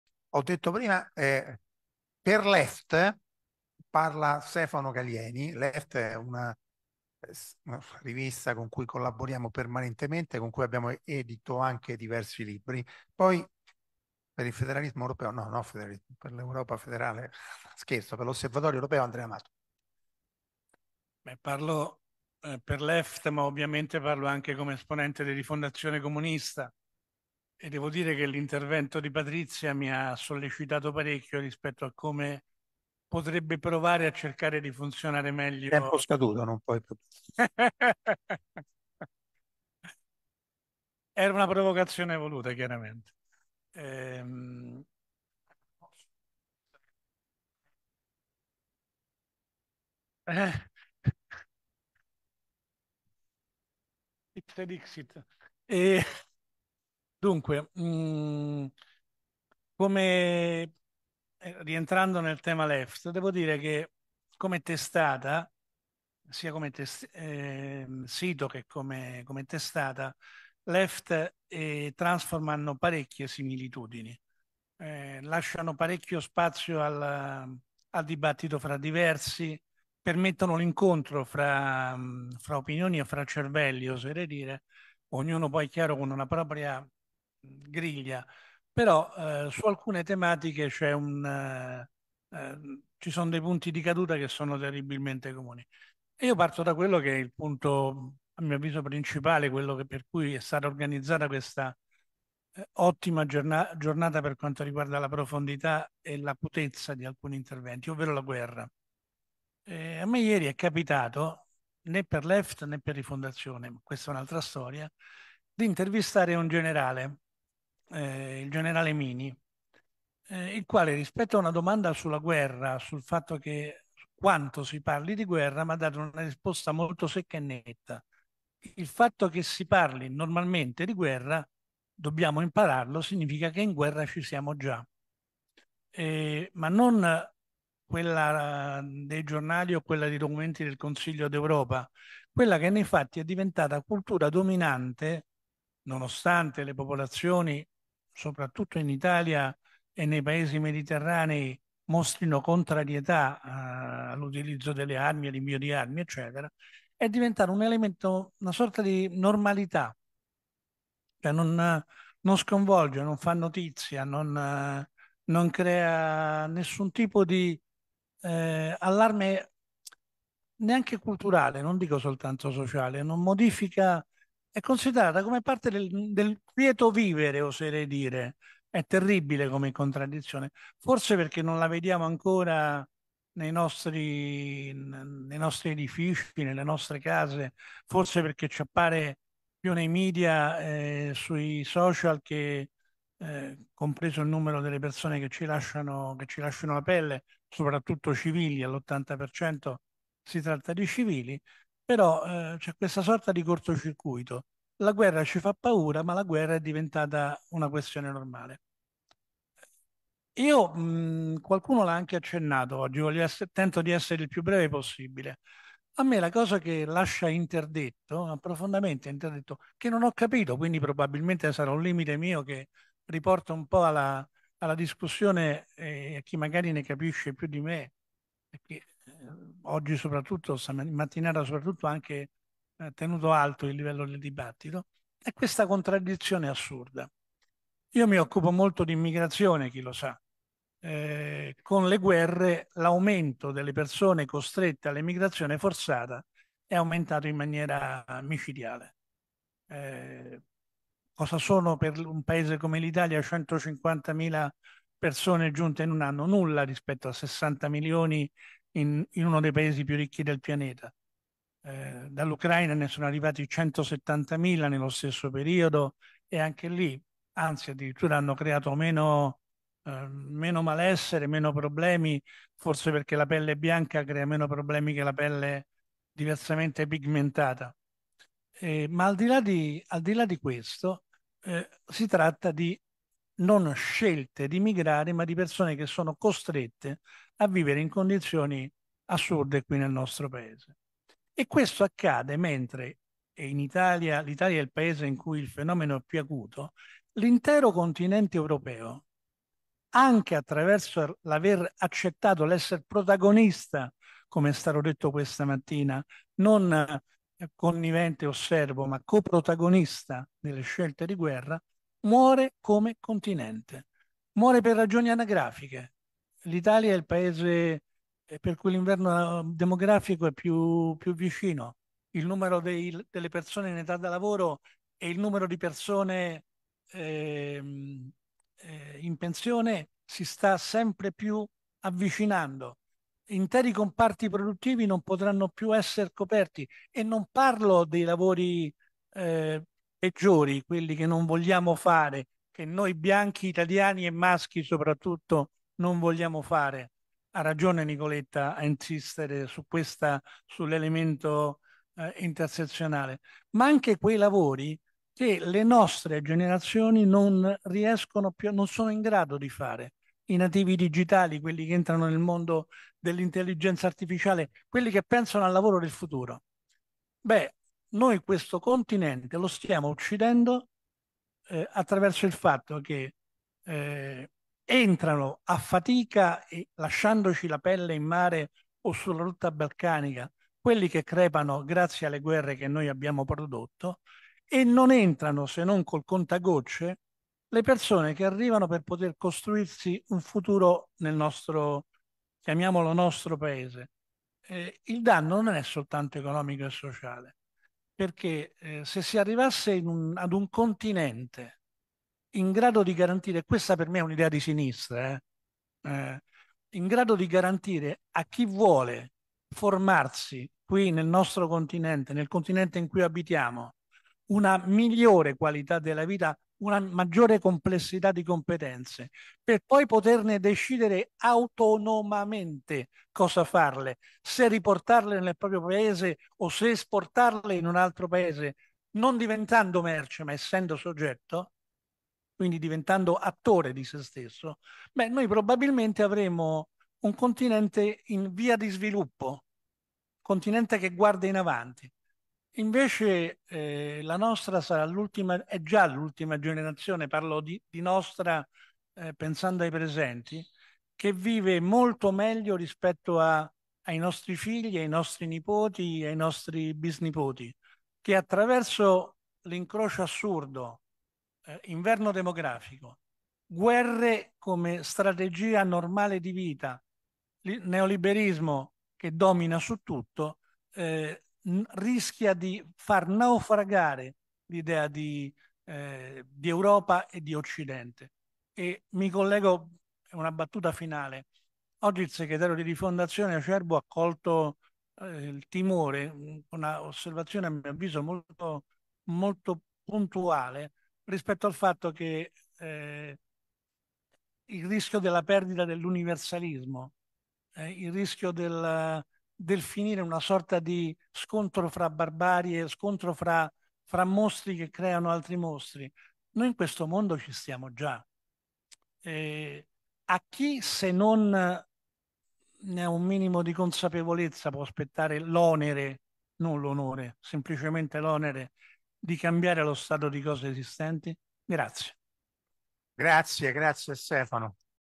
assemblea 2 07 Galieni
Sessione pomeridiana dell'assemblera annuale di transform!italia che si è tenuta sabato 23 marzo 2024 alla Casa  Internazionale delle Donne di Roma.
7) Stefano Galieni